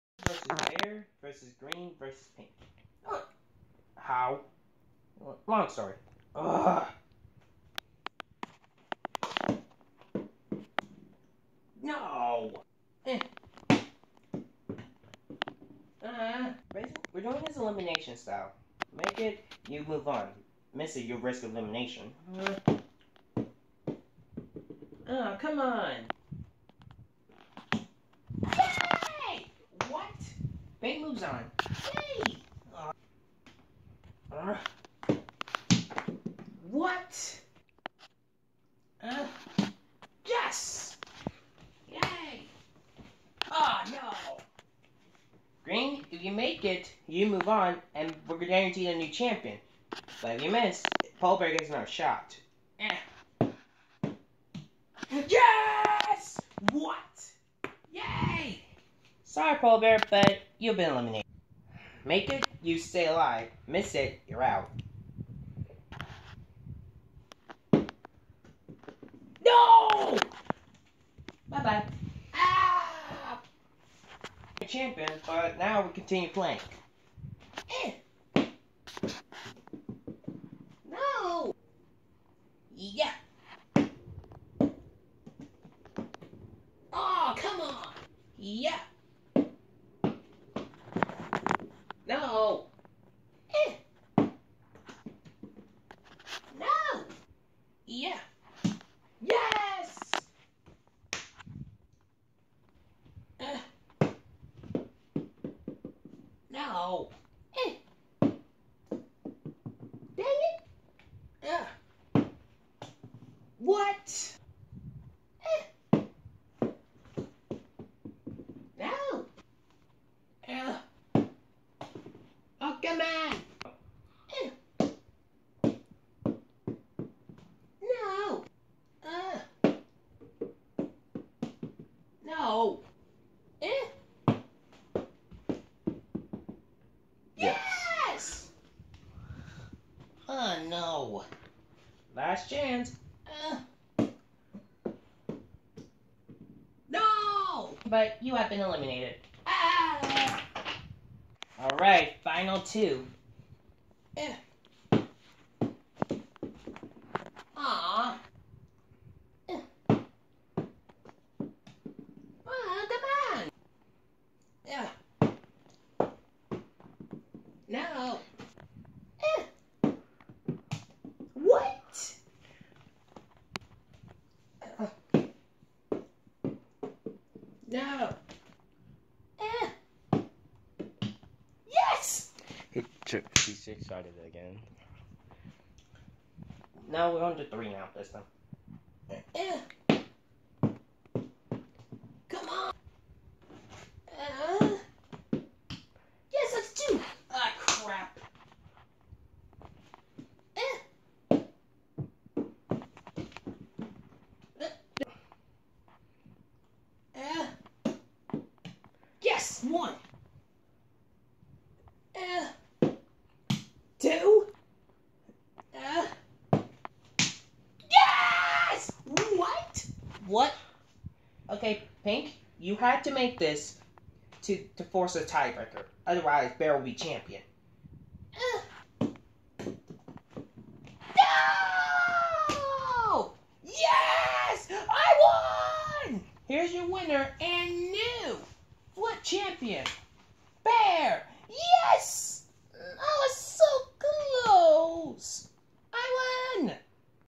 Flip fatal 4 way match
Polar Bear (Flip champion) vs Bear vs Green vs Pink for the flip championship